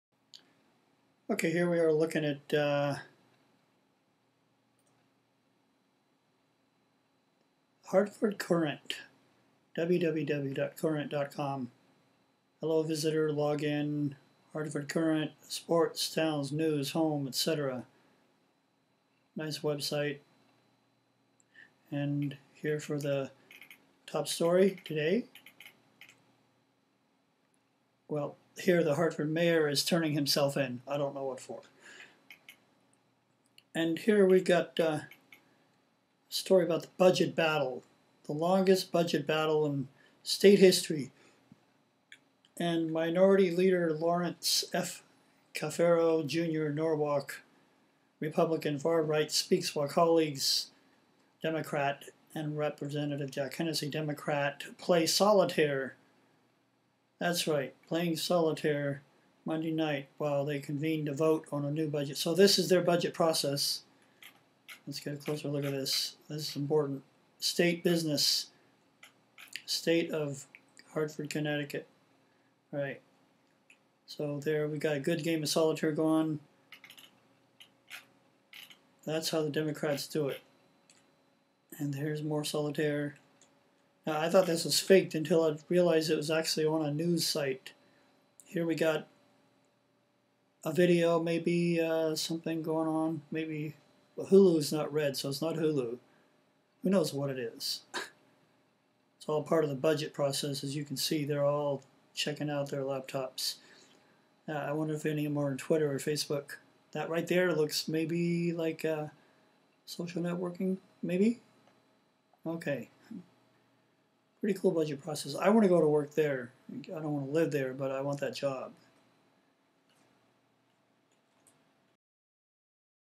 Solitaire Is Integral Part of Connecticut Budget Process Sep 1, 2009
From the Hartford Courant, Connecticut Democrat lawmakers play solitaire while Republican minority leader addresses the floor.